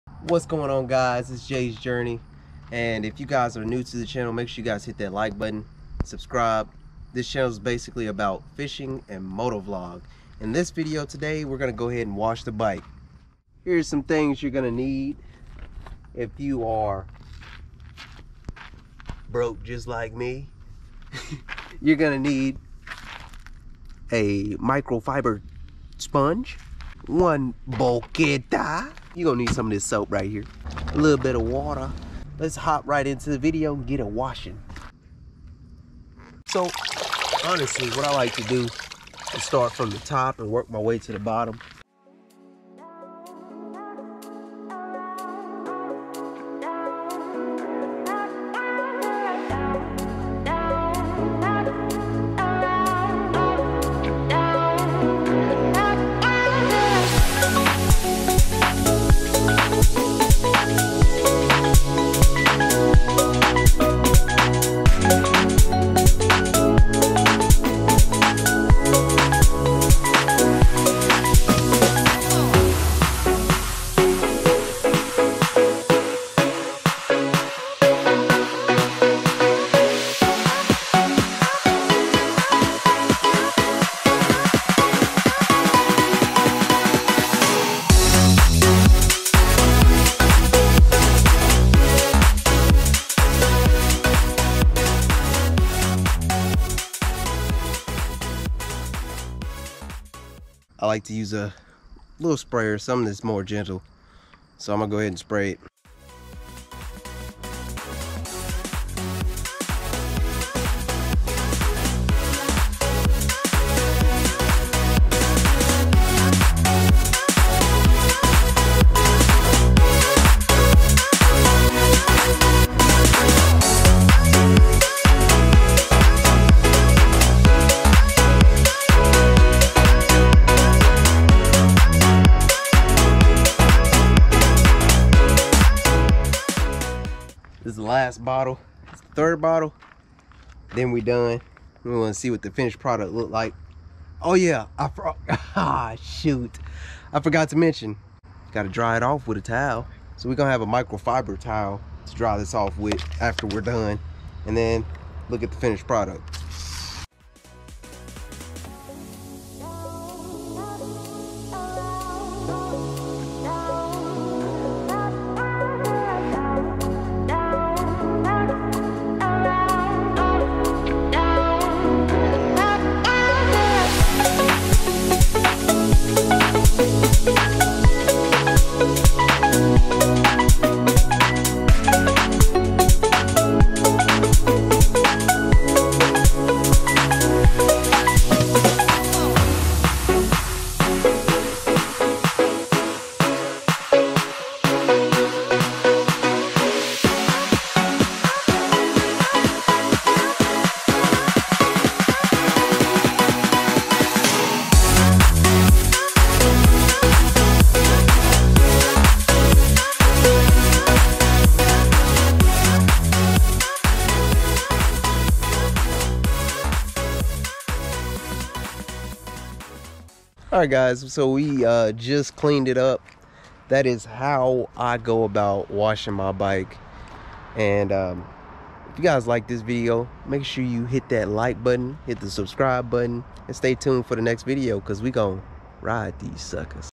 How To Clean Your Bike 5X BETTER
In this video i show you how I clean my bike and what products I use on it. How often do you clean your bike? Do you use a pressure washer if so how effective is it? Let’s go ahead and jump right into the video.